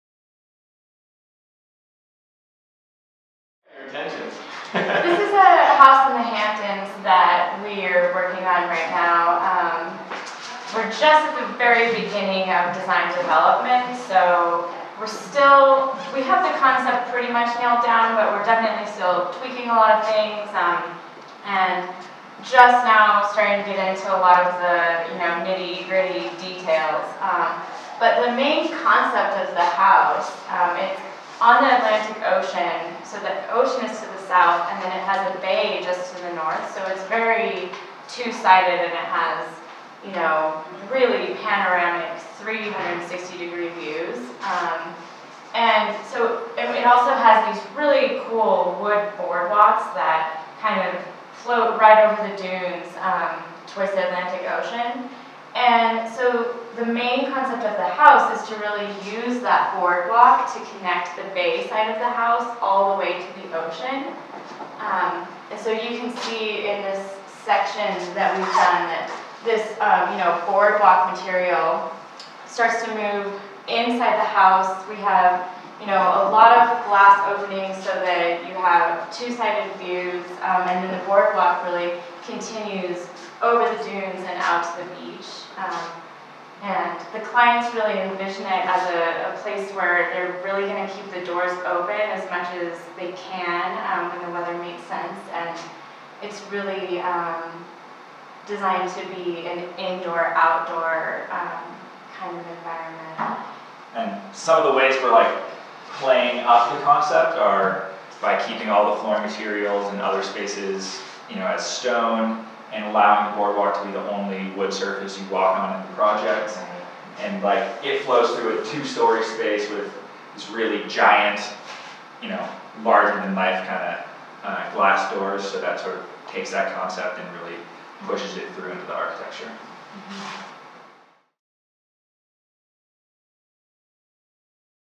AIA Colorado West - CCY Architects Southampton, NY Project
Colorado Architecture Month 2016 helps you get to know AIA Colorado West with CCY Architects